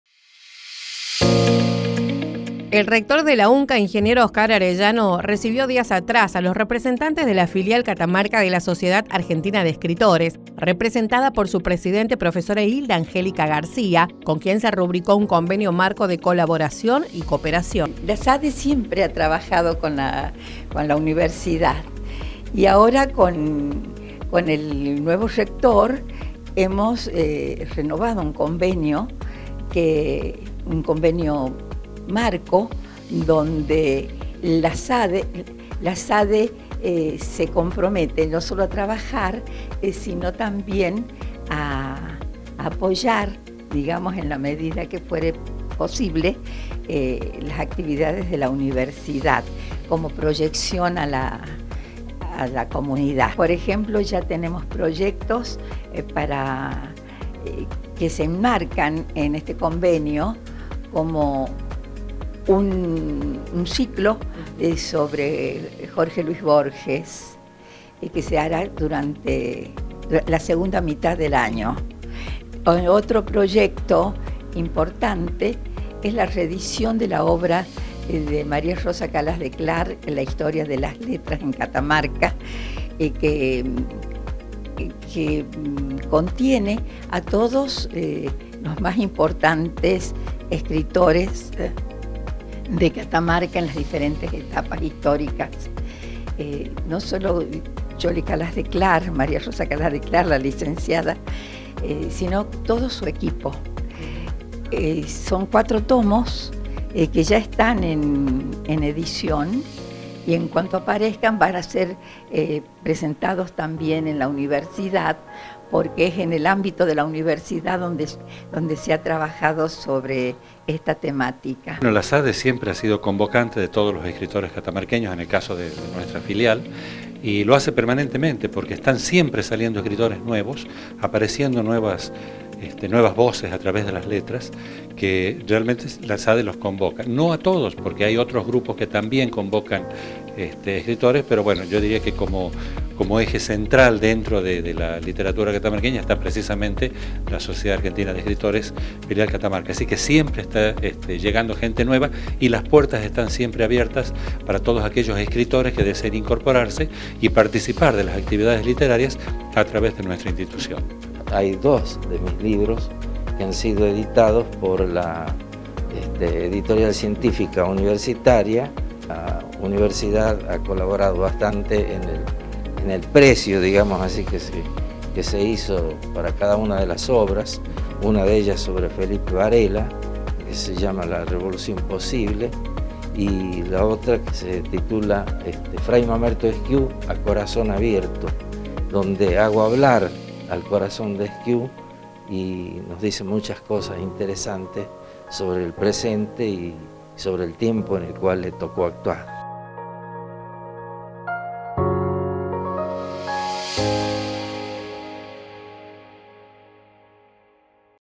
LA UNCA RENUEVA CONVENIO CONLA SOCIEDAD ARGENTINA DE ESCRITORES
El rector de la UNCA, Ing. Oscar Arellano, recibió días atrás a los representantes de la filial Catamarca de la Sociedad Argentina de
Escritores, representada por su presidente, Prof. Hilda Angélica García, con quien se rubricó un Convenio Marco de Colaboración y Cooperación.